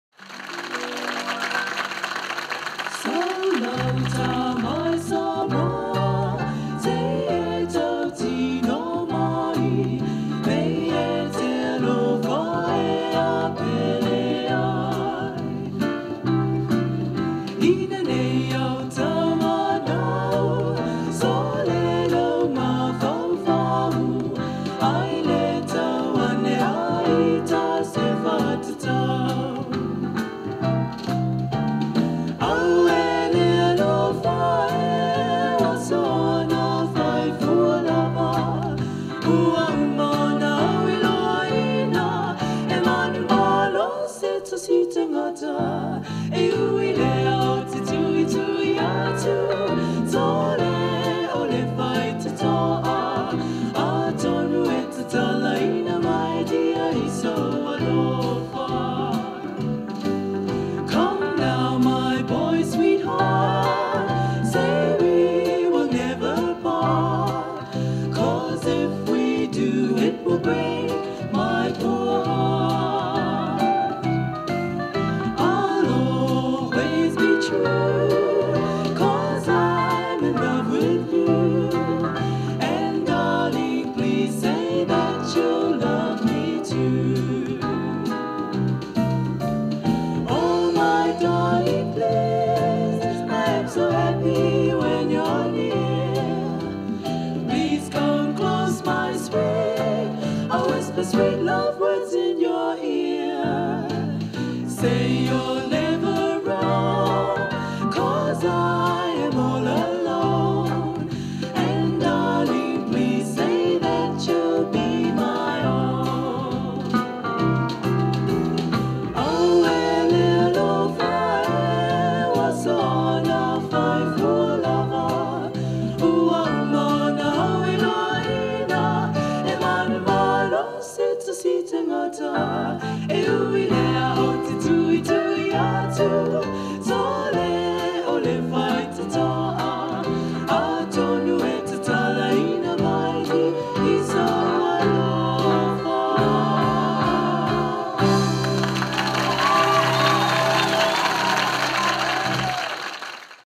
The Three T's - live TV appearance 1984!!
The Three T's - Tui, Tina and Timu - were an immensely popular act on the South Auckland nightclub circuit in the early 1980's, with sweet Pacific harmonies and songs of home (Samoa) ringing true for many thousands of Polynesians who flocked to clubs like Cleopatras, the Peppermill and others to catch their midnight floorshows...a rare find for the Slydogmania channel